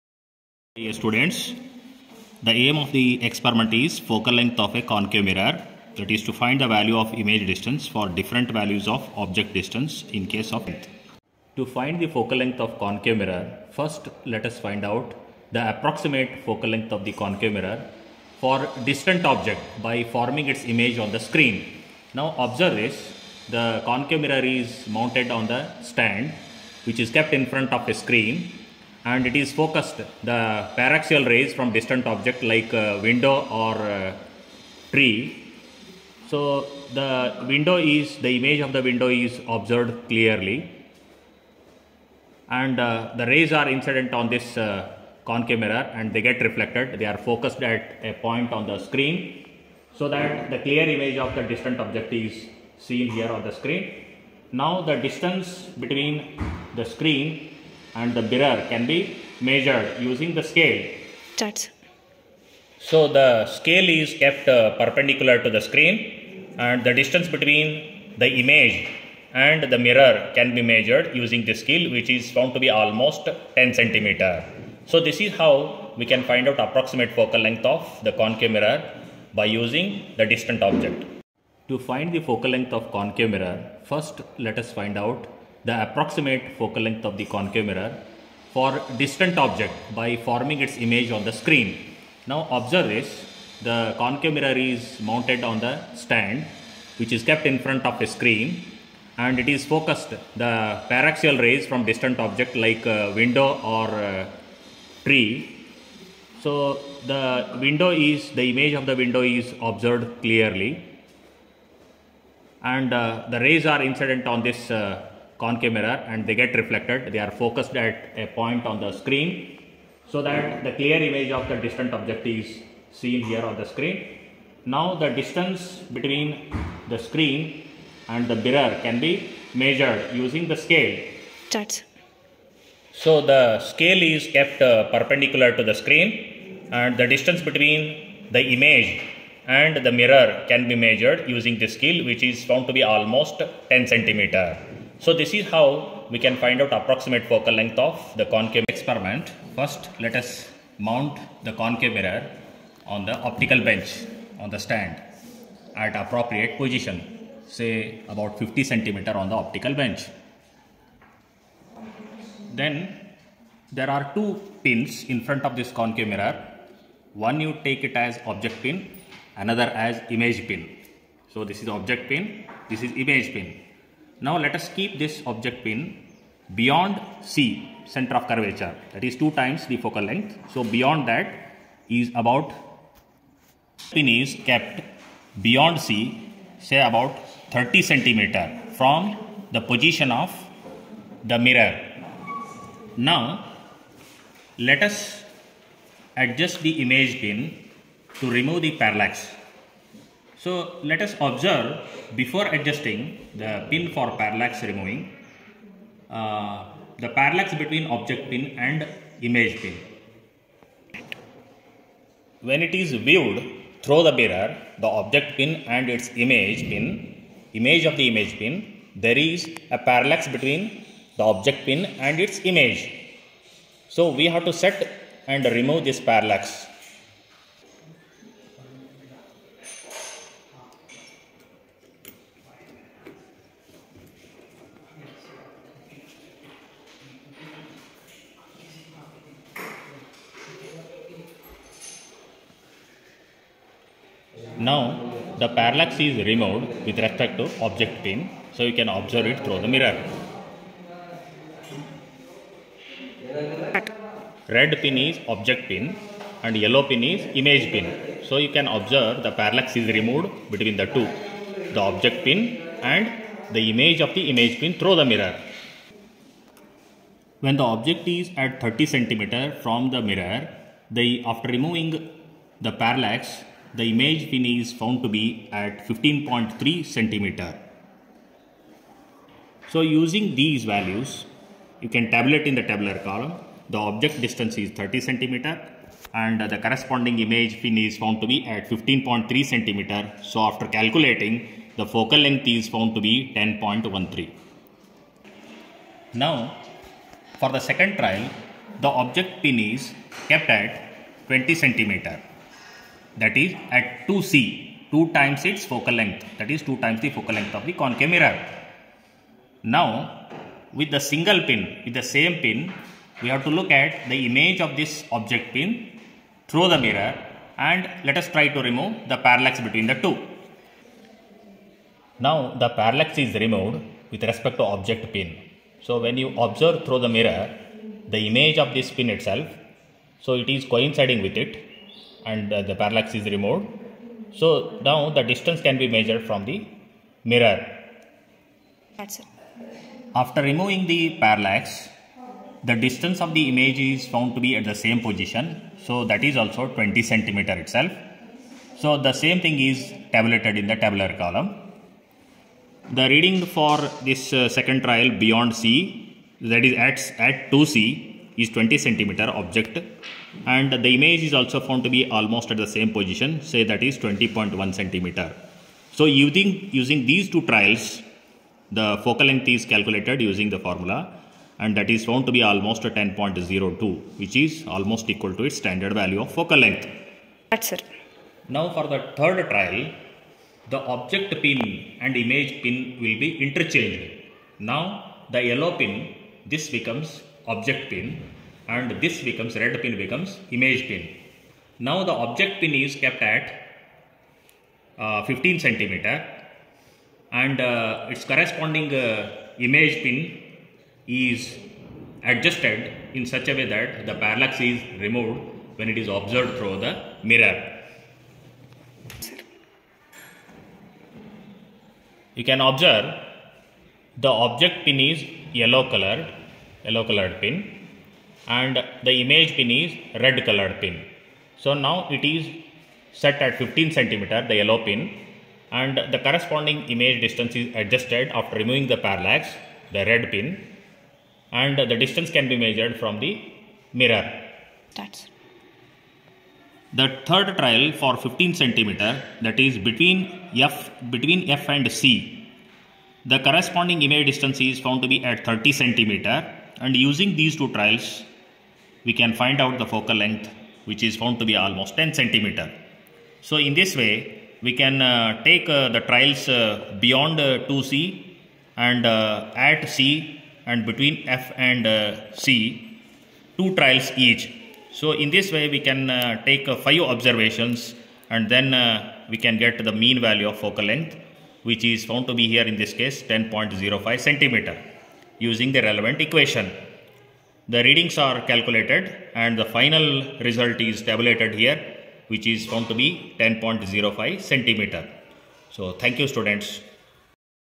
Focal length of concave mirror
To find the focal length of concave mirror by parallax method